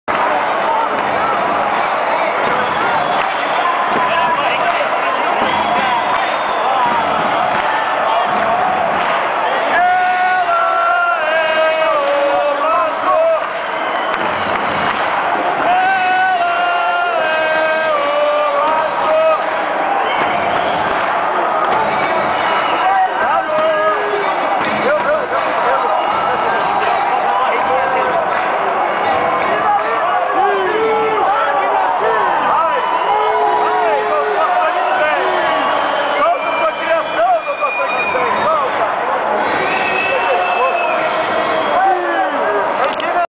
Edinanci Silva chegando para lutar a final do Pan Rio-2007
Edinanci Silva chegando para lutar a final do Pan Rio-2007. Imagens de uma camera amadora da arquibancada. Categoria -78KG